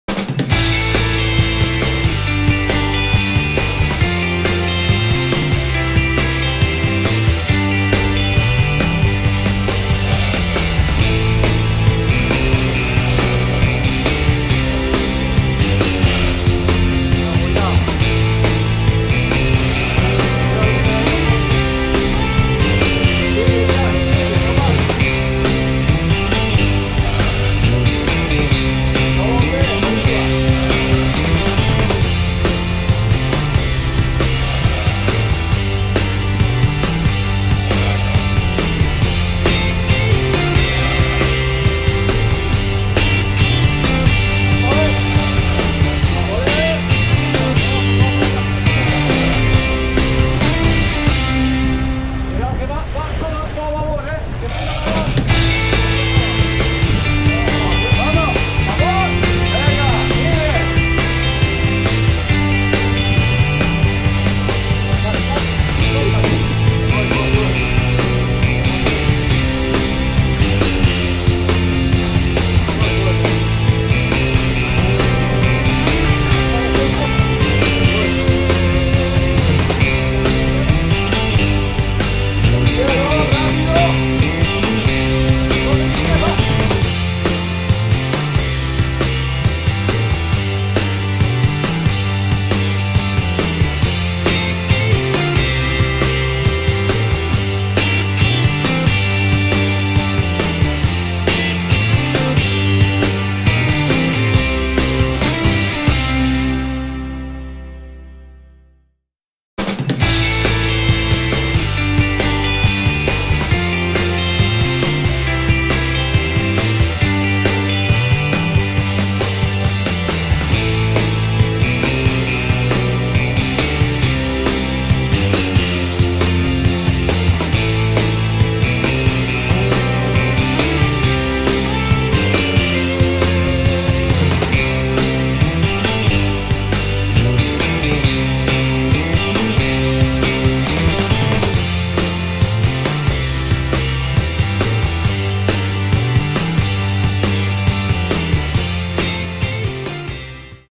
Big Eye Tuna early July 2020 in La Gomera with Maynieves
Fishing is GREAT in La Gomera at the moment. We're catching blue marlin, white marlin, big eye tuna and yellow fin tuna these days. Capt. Gianmaria sent us this great video from a big tuna caught last week.